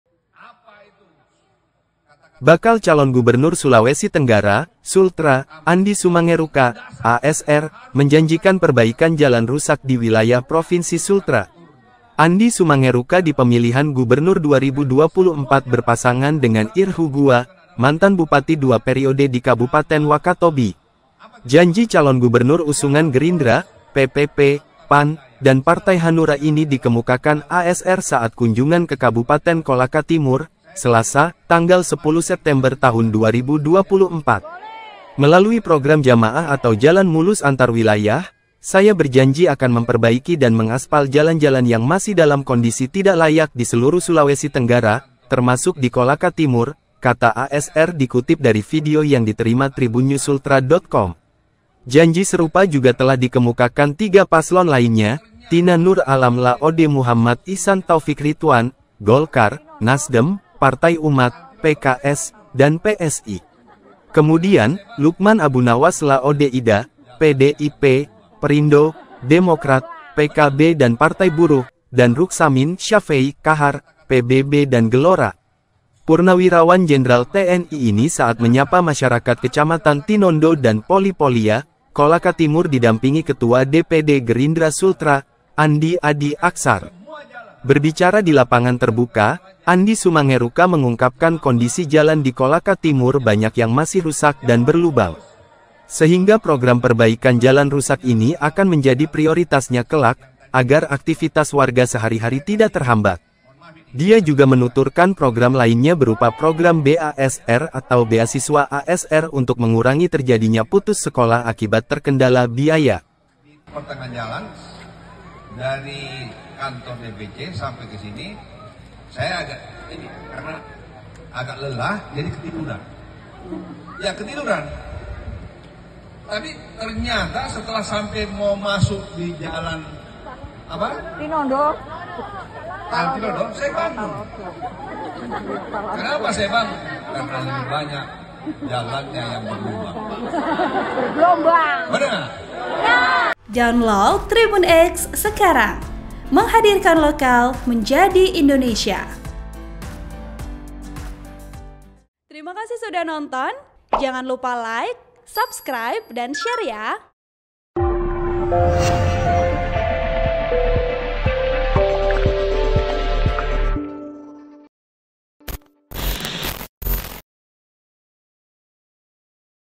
Andi Sumangerukka Hugua Janjikan Perbaikan Jalan Rusak di Sultra saat Kunjungan ke Kolaka Timur
TRIBUNNEWSSULTRA.COM, KENDARI - Bakal Calon Gubernur Sulawesi Tenggara (Sultra), Andi Sumangerukka (ASR) menjanjikan perbaikan jalan rusak di wilayah Provinsi Sultra.

Andi Sumangerukka di Pemilihan Gubernur 2024 berpasangan dengan Ir Hugua, mantan bupati dua periode di Kabupaten Wakatobi.

Program: Pilkada Sultra
Video Editor: Rheymeldi Ramadan Wijaya
Sumber: Dewi Lestari
Uploader: